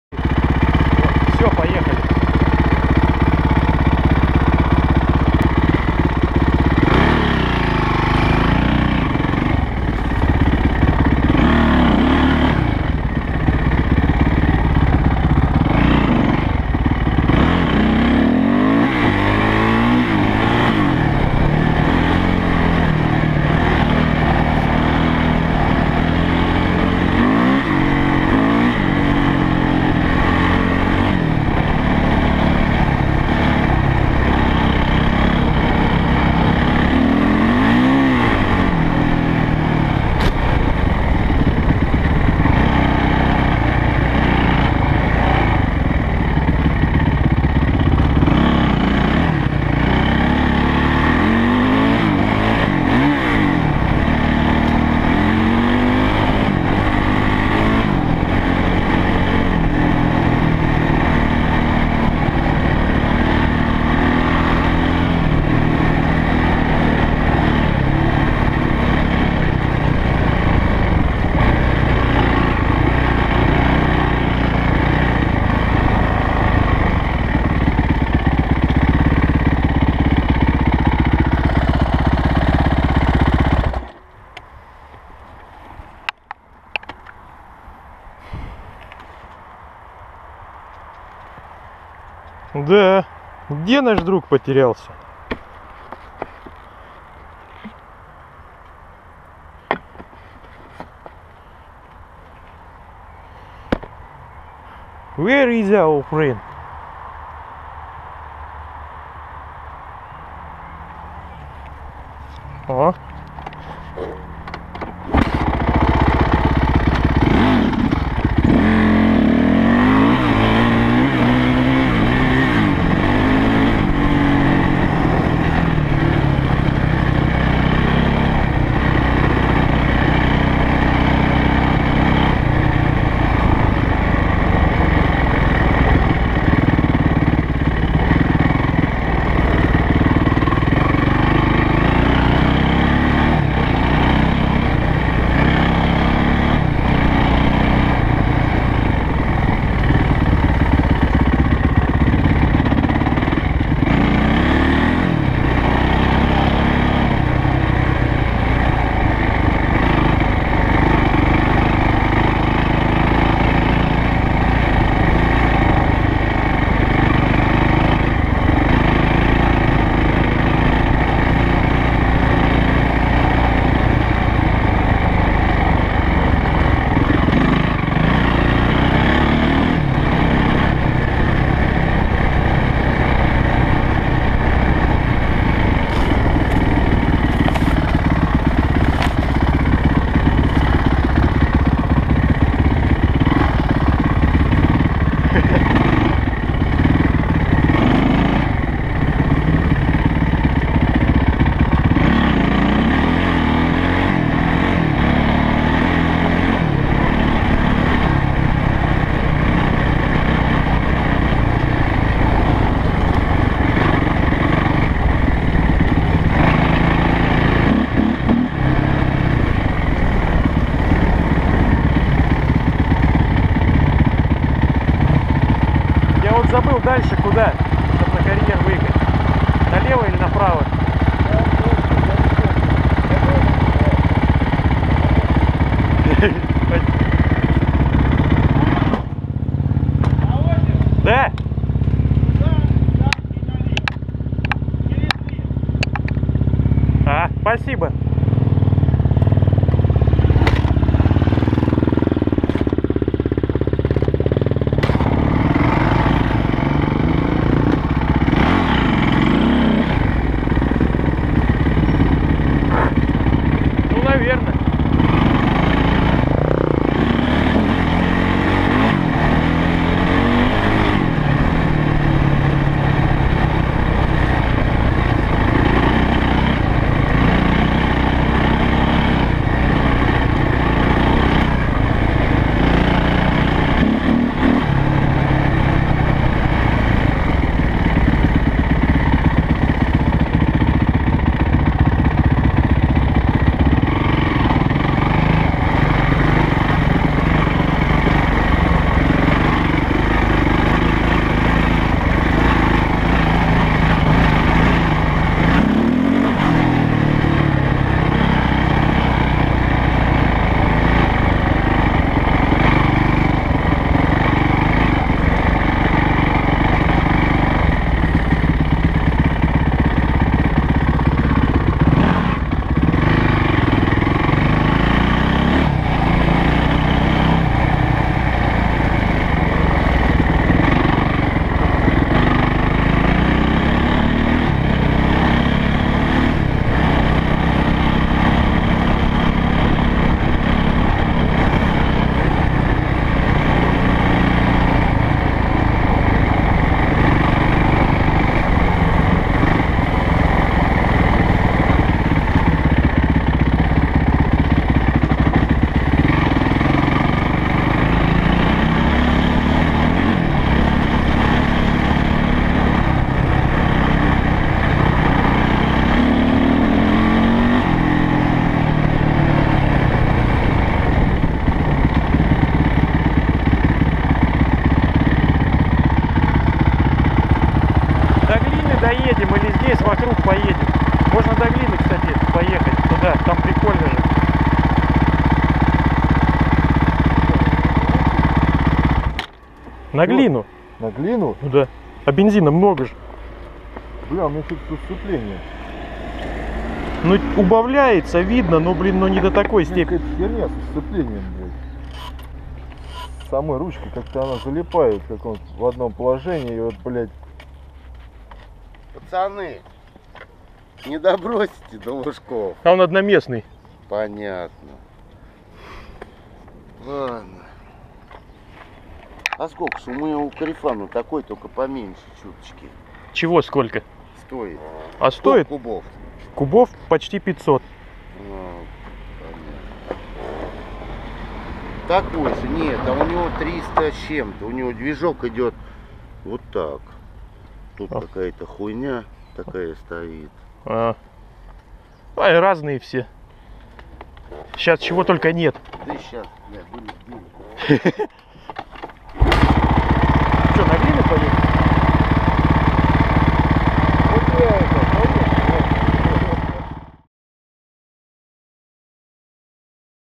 Yamaha YZ450F First ride 3 Field road
Hi guys! This is my first serious ride on my bike! This was awesome!